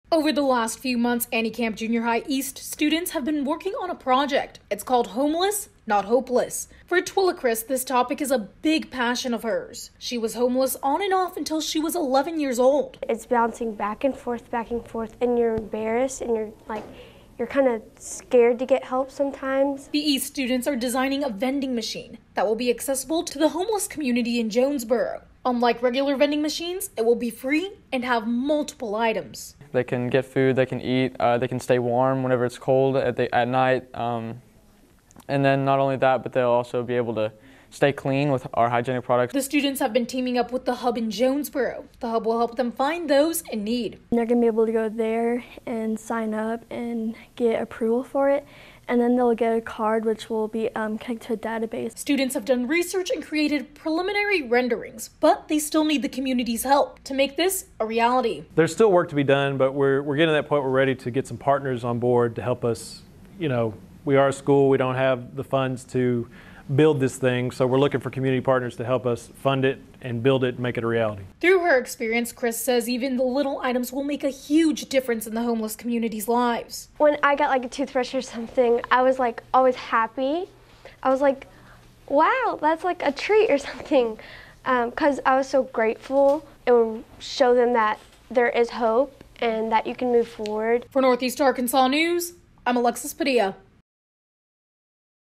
Homeless Not Hopeless EAST Project
There are more than 350 homeless people in Jonesboro, according to the HUB. Annie Camp Junior High’s E.A.S.T. program is trying to do something to help. Northeast Arkansas’ Alexis Padilla tells us how.